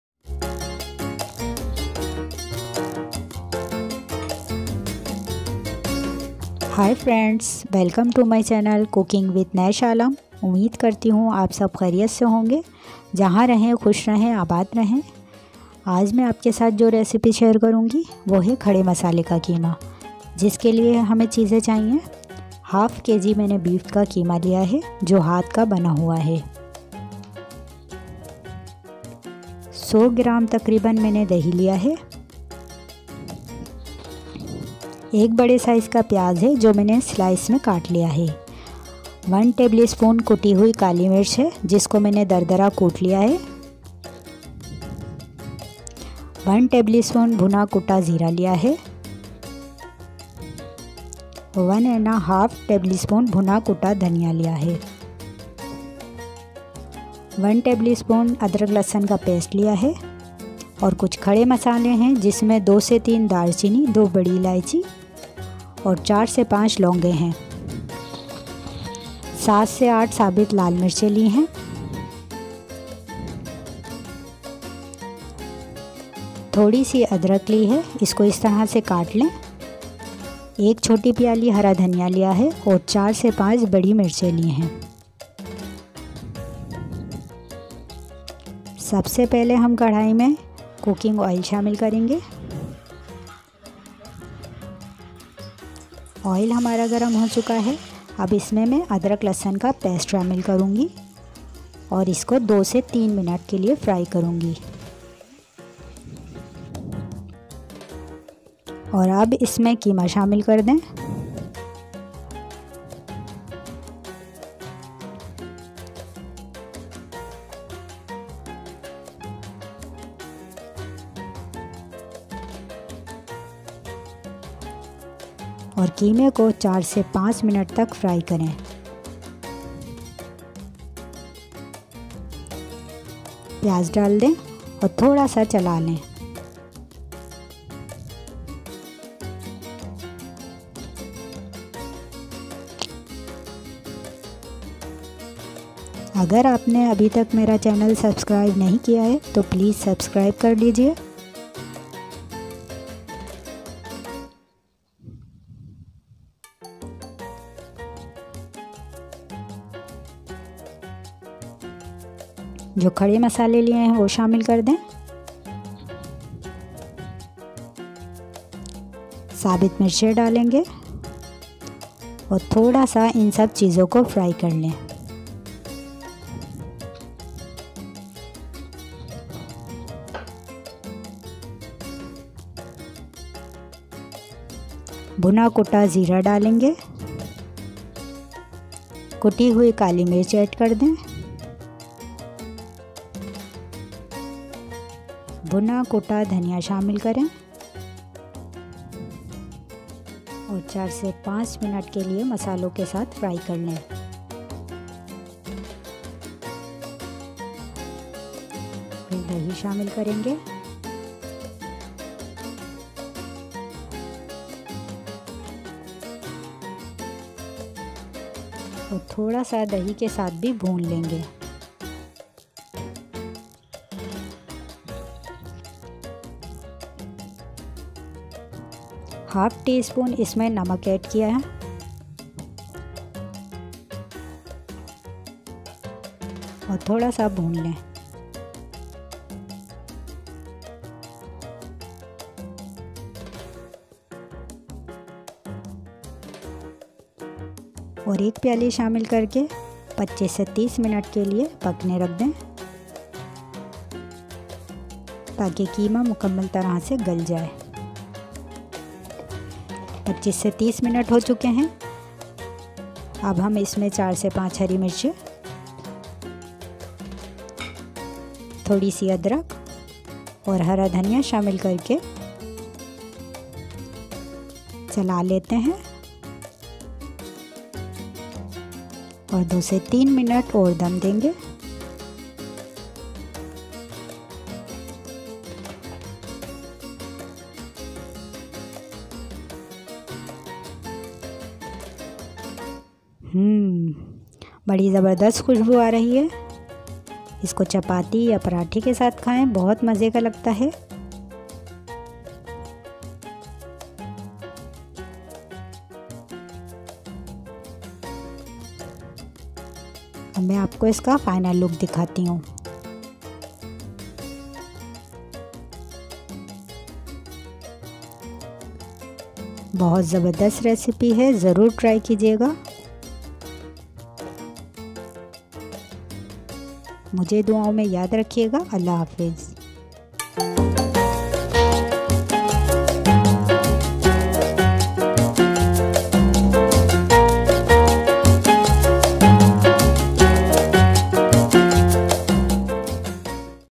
Khaday masalay ka fry keema |Qeema Fry |کھڑے مصالہ کا قیمہ فرائی |Bhuna huwa Qeema
السلام و علیکم آج کی ریسیپی ہے کھڑے مصالہ کا fry قیمہ ۔ جو کہ نہایت آسان اور مزیدار recipe ہے امید ہے آپ کو پسند آئیگی ۔ اگر آپ کو میری ریسیپی اچھی لگے تو like اور share کریں ۔ اور میری ویڈیو کا notification کے لئے چینل کو sunscribe کریں
شکریہ